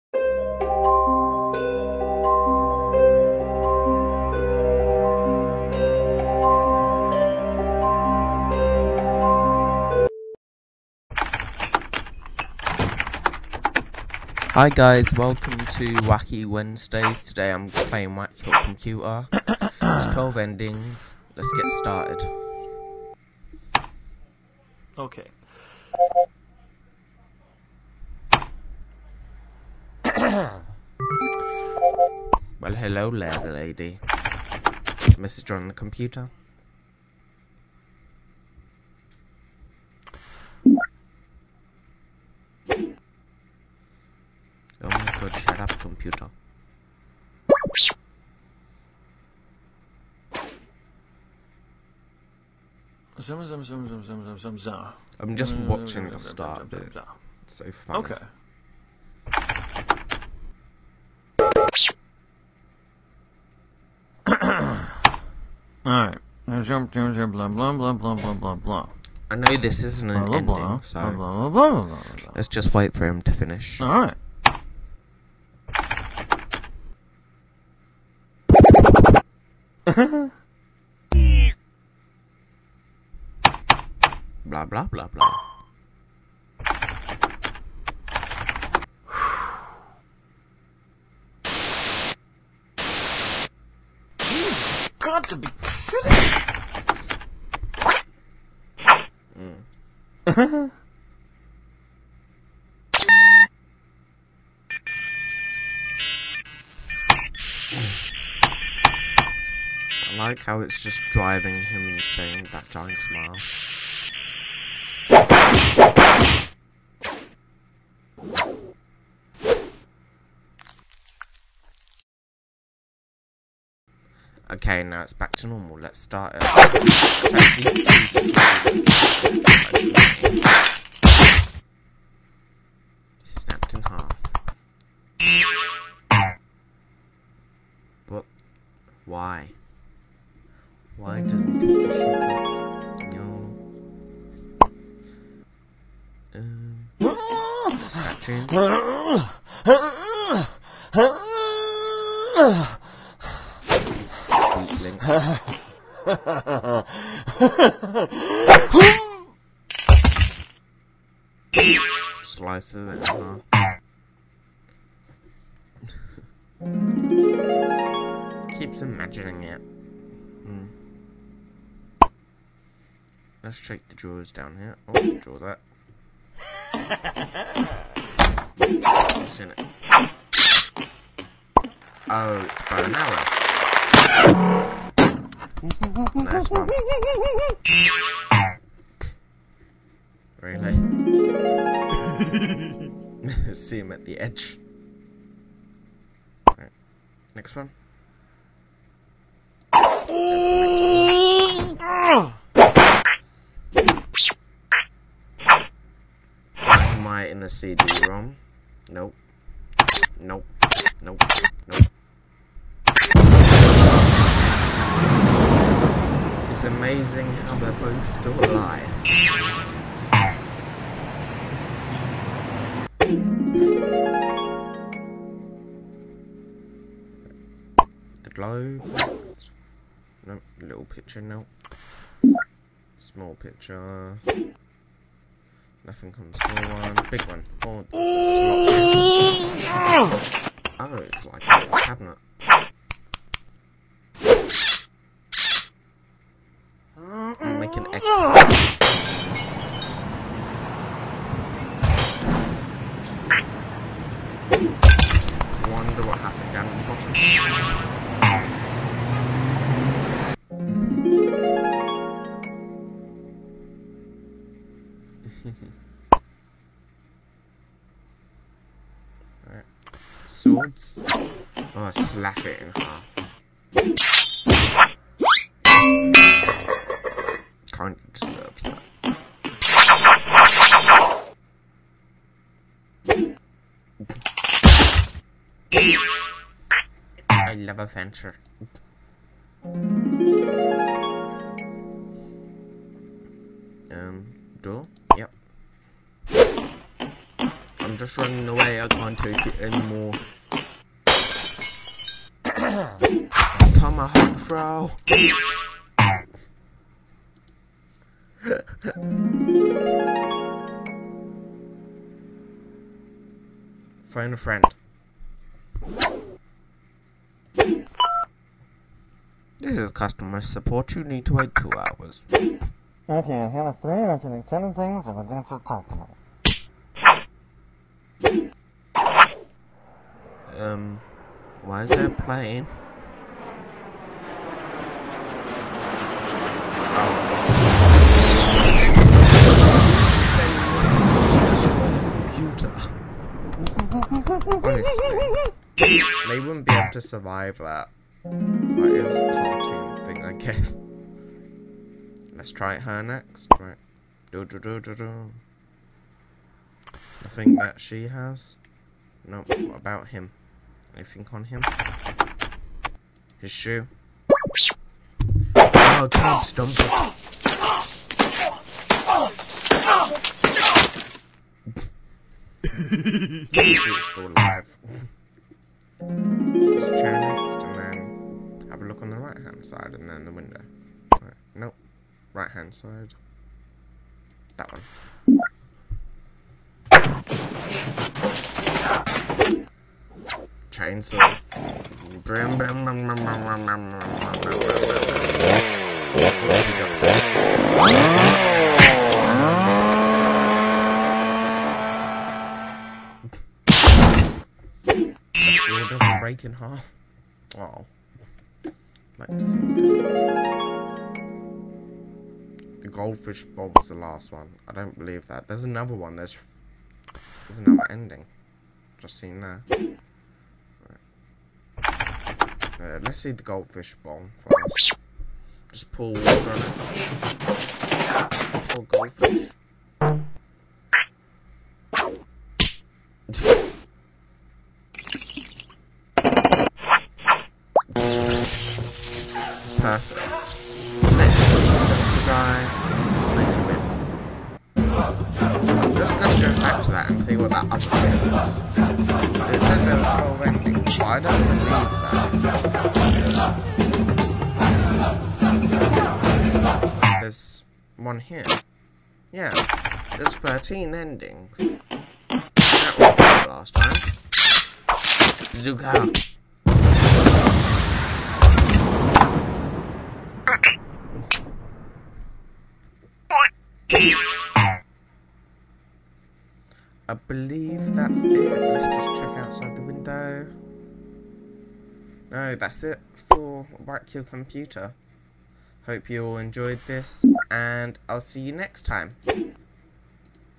Why you facebook meh? Whacky Wendsdays
Hello my enders today video is whack your computer i hope you all enjoy :)